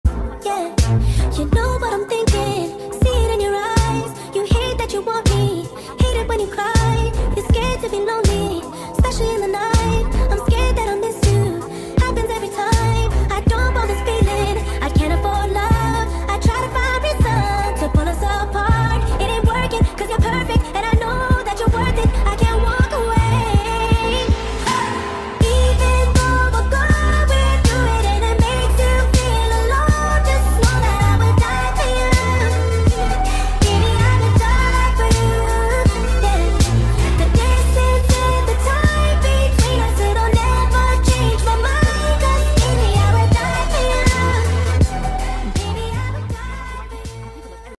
look like a song by the weeknd 🎤
this was a request made by goldfish! if you have any requests please comment!

affirmations

“i look like a song by the weeknd. people are reminded of songs by him when they see me. i am hot and mysterious. i am confident in myself. many people admire me. i have my desired features. i have pretty privilege. everyone is mesmerized by me.”